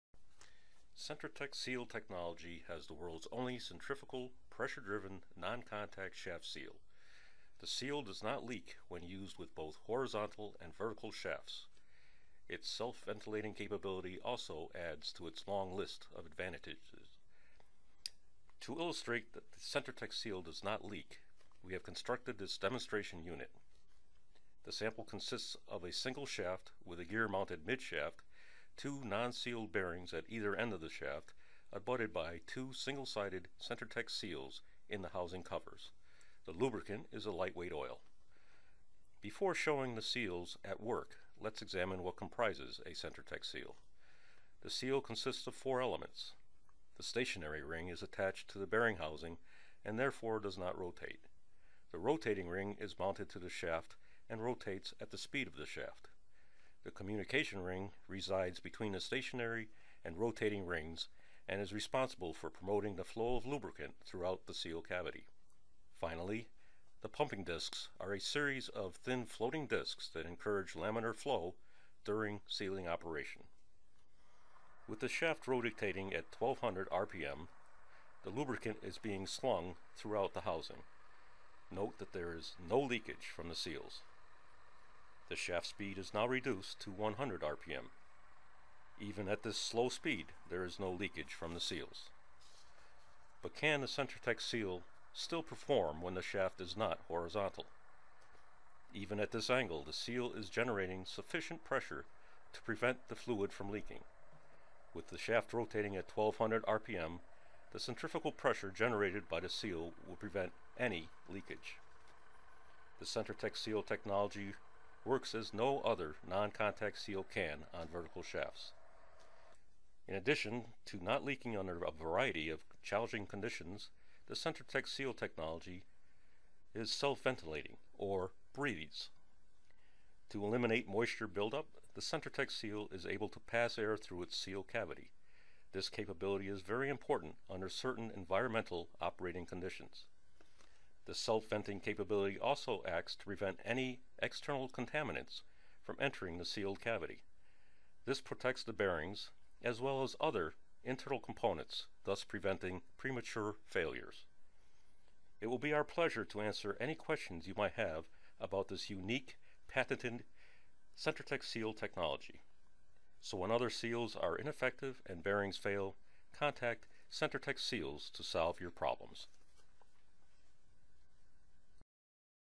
Non-Contact Shaft Seals – Centritec from Carlyle Johnson
Centritec Seals are the world’s only centrifugal, pressure-driven, non contact shaft seal. They reduce downtime and maintenance by eliminating lubricant leakage and keeping contaminants out of the seal. Designed for both horizontal and vertical shafts, these non-contact shaft seals are ideal for extreme environments including turbines, rolling mills, railroad cars and locomotives, off-road construction vehicles and wind power generation.